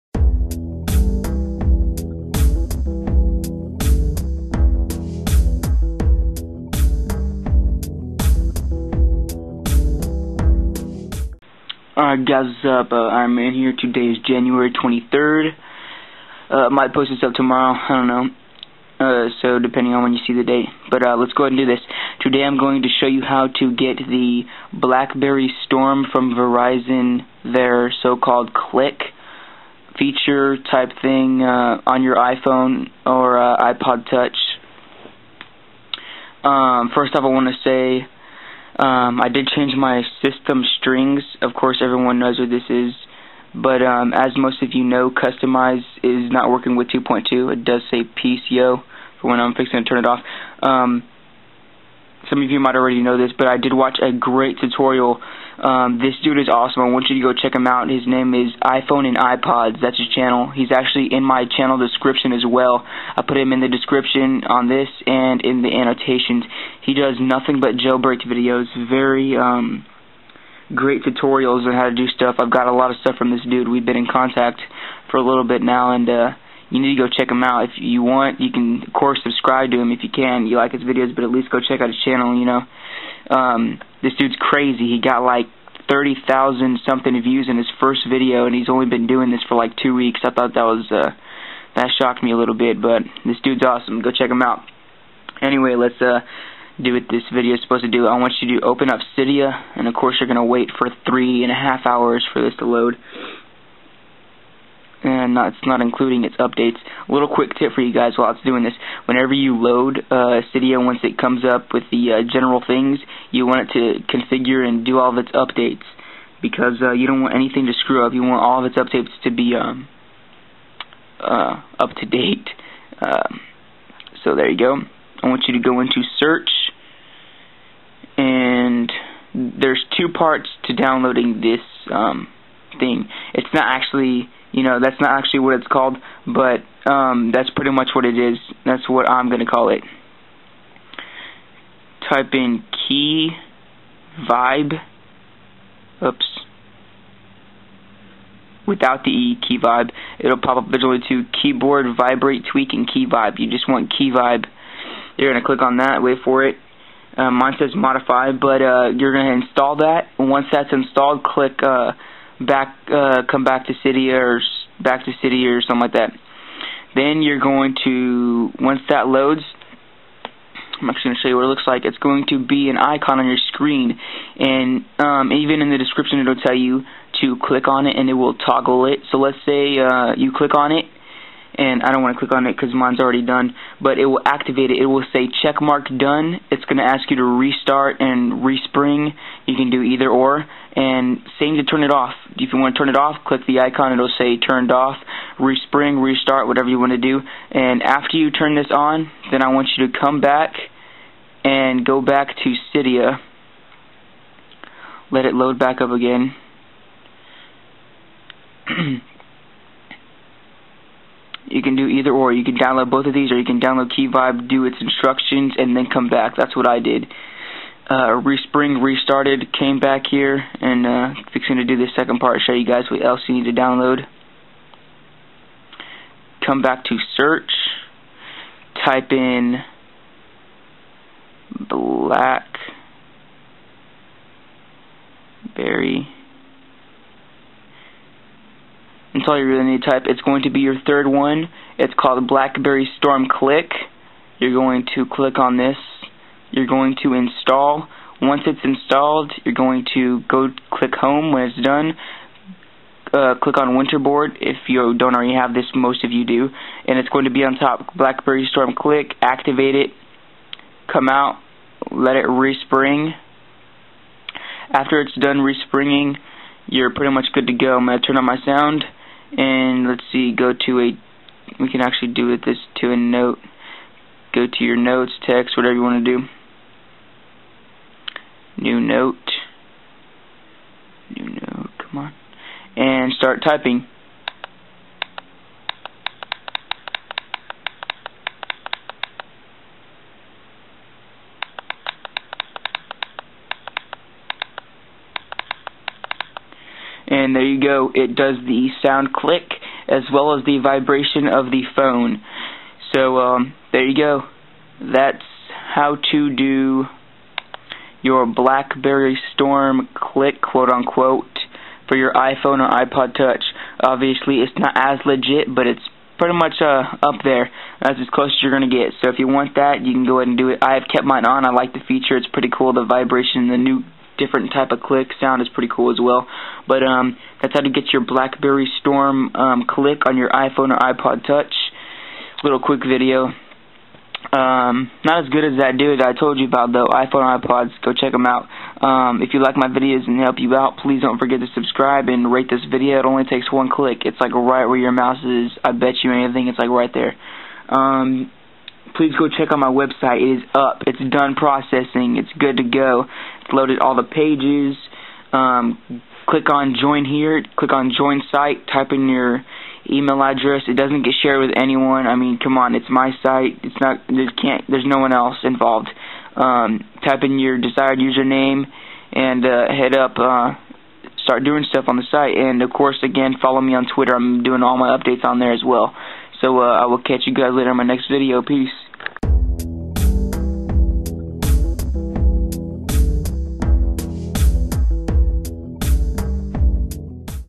How to get BlackBerry Storm 'Click' keys on your iPhone/iTouch
Website is up!

MUST have a Jailbroken device!
This is a video tutorial on how to get the BlackBerry Storm 'Click' on your iPhone and or iPod touch.
My version of it anyway..

NO sources need to be added in order to get this, as said in the video, but you need to download two applications

1. Keyvib
2. Blackberry Storm Click

Install 1 is optional, but highly recomened inorder to get that blackberry storm feel, well atleast for the iPhone.

ironman333333